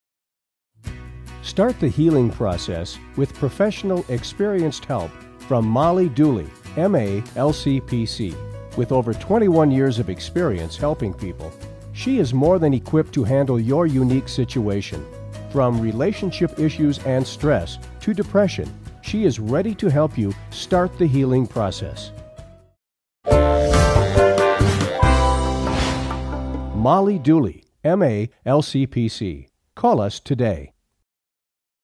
Dooley Molly Ma Lcpc
ma_lcpc-b150983

Start the healing process with professional, experienced
help from Molly Dooley, MA LCPC. With over 21 years of
experience helping people, she is more than equipped to
handle your un